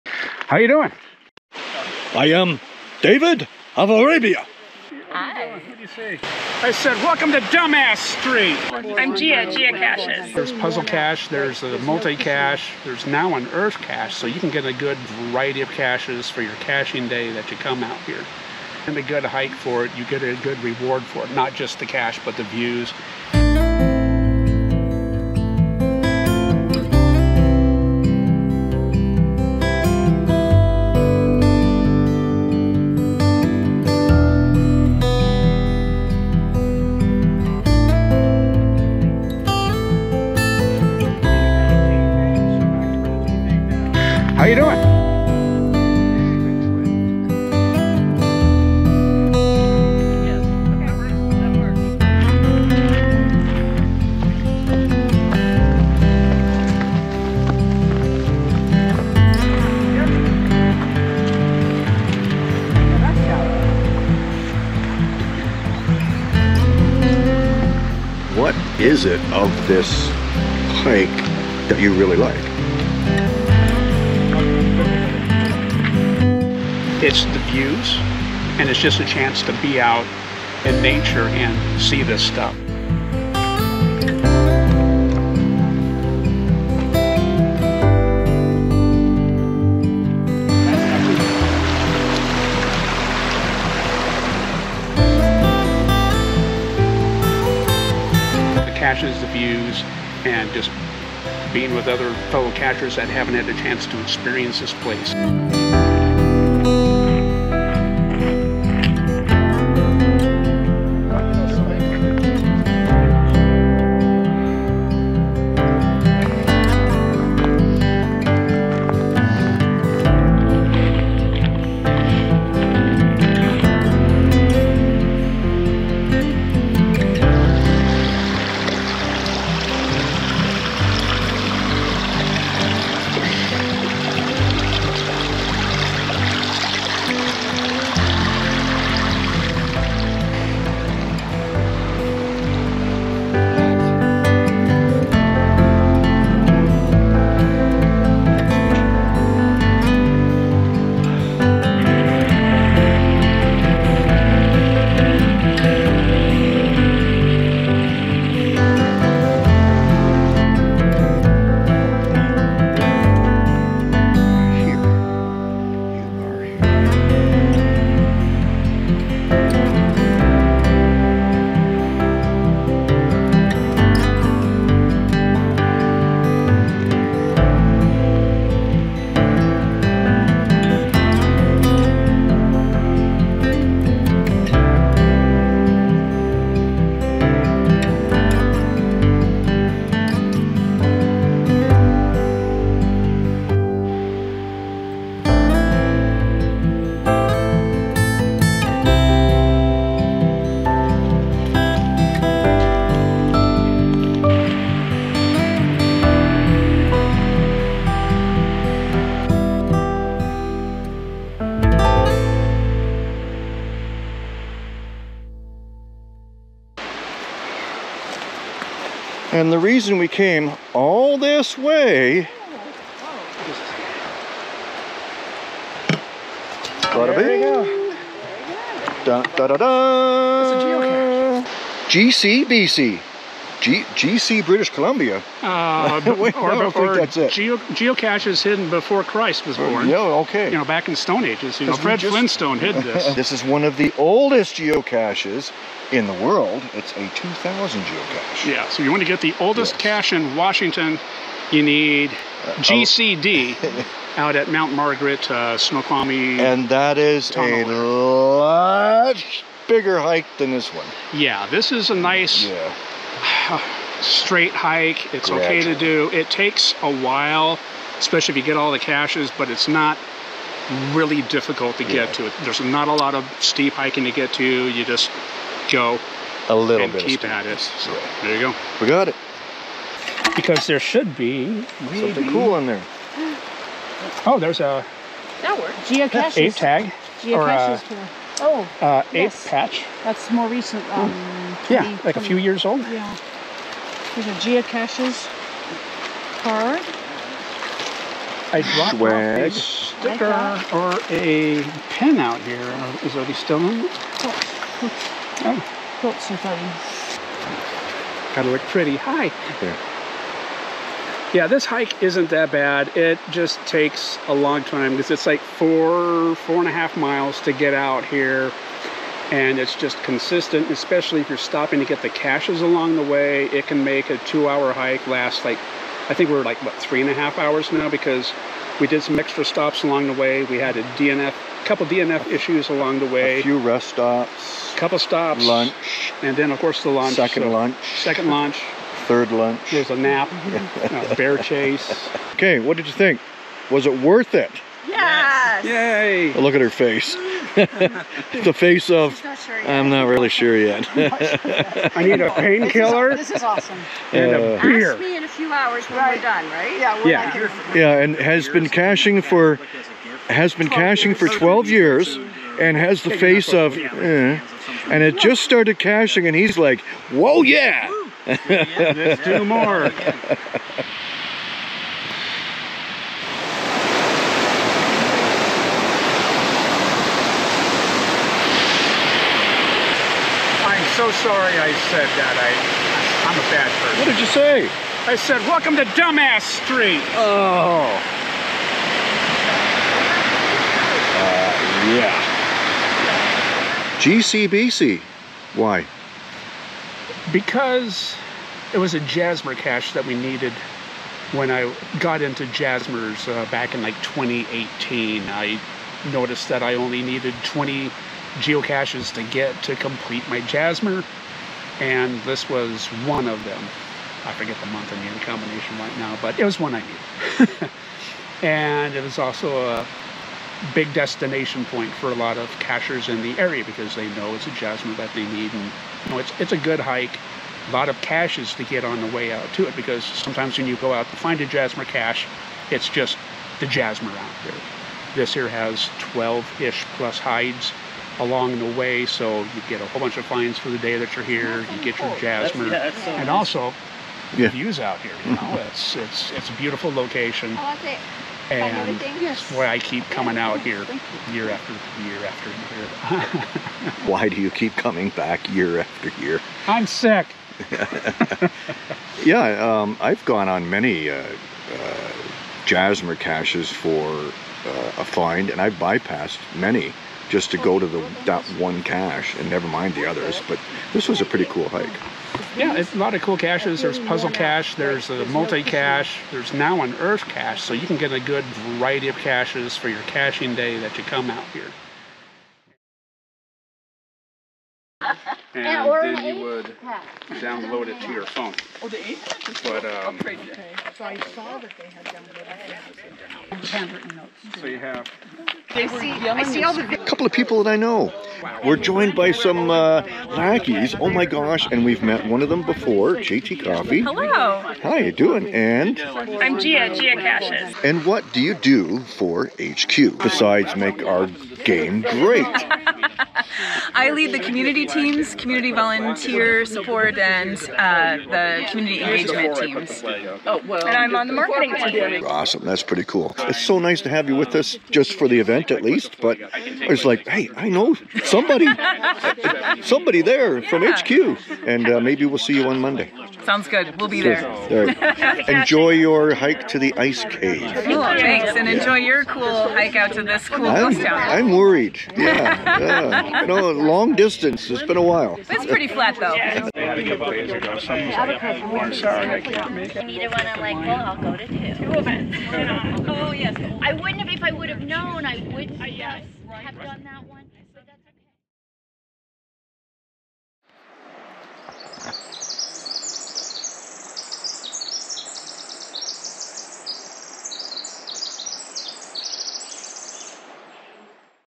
Geocaching Monte Cristo
Come on a hike with us to the abandoned town of Monte Cristo in Washington state and the associated geocache. We start with a musical journey celebrating the trail to the town then find the geocache and talk with YouTuber Komikino about the hike and what it means to him.